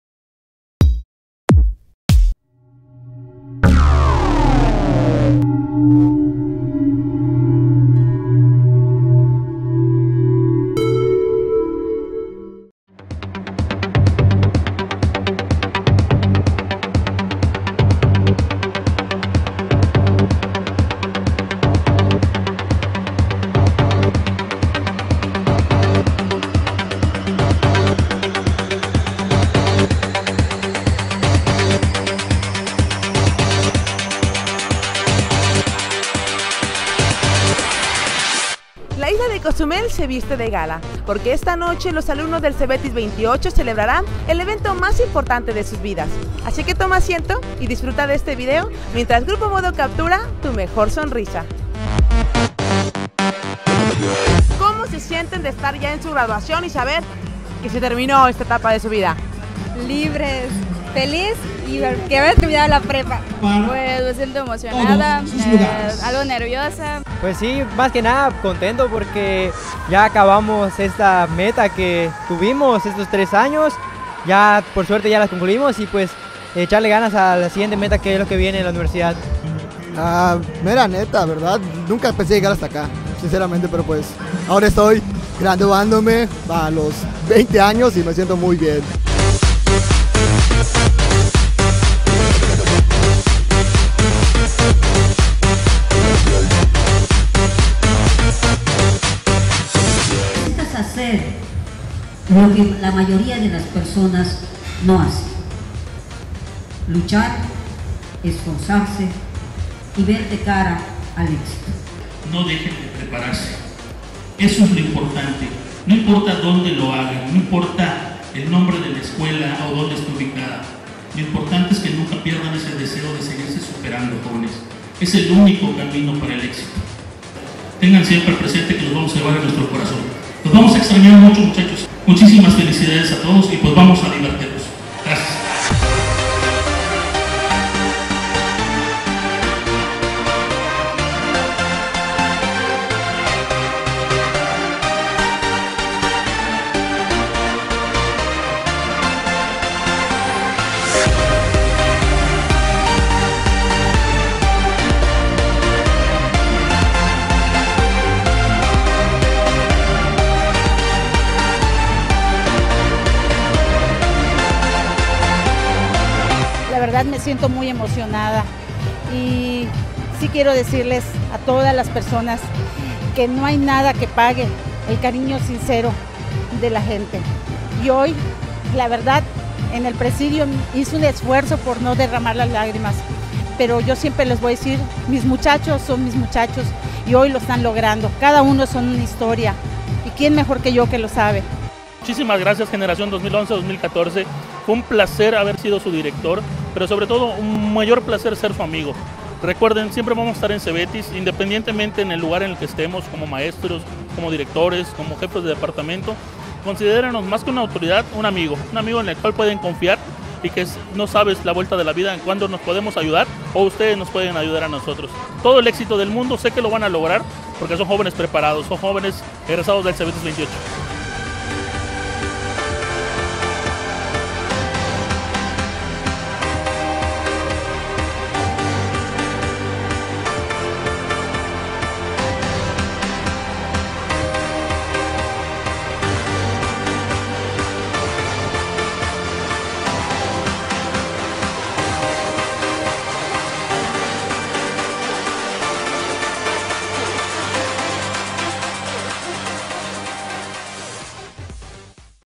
DGETI QUINTANA ROO PLANTEL CBTIS 28
VIDEO MEMORIA DE LA CEREMONIA DE GRADUACIÓN DE LA GENERACIÓN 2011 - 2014 DEL PLANTEL CBTIS 28 COZUMEL